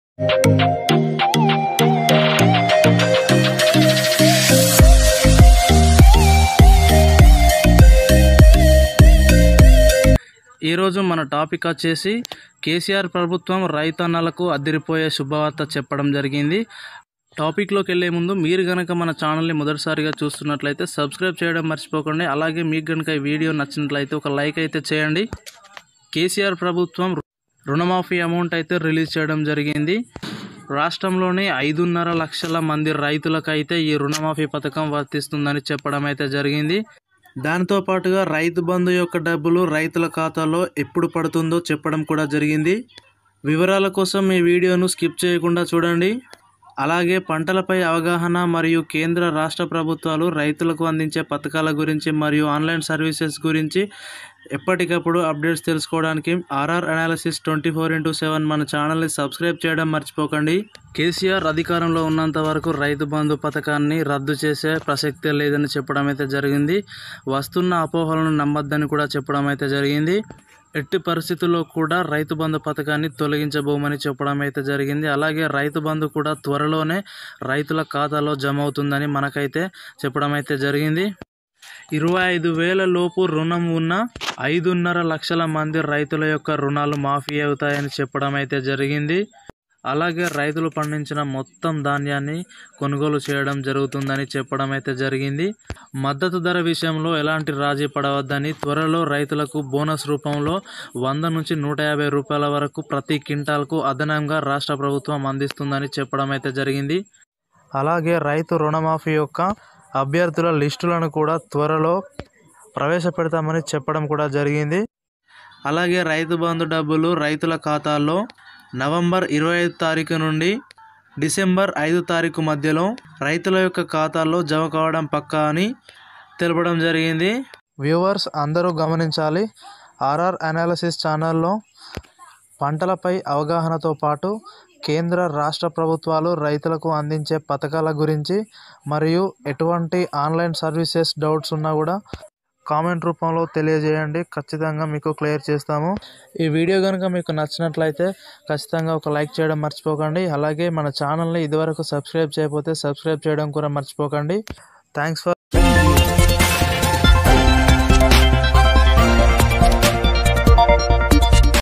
తెలంగాణ రైతన్నలకు రైతు బంధు అద్దిరిపోయే శుభవార్త | రుణమాఫీ జిల్లాల వారీగా ఖాతాలో జమ | Rythu Samachar
తెలంగాణ రైతన్నలకు రైతు బంధు అద్దిరిపోయే శుభవార్త | రుణమాఫీ జిల్లాల వారీగా ఖాతాలో జమ | Rythu Samacharam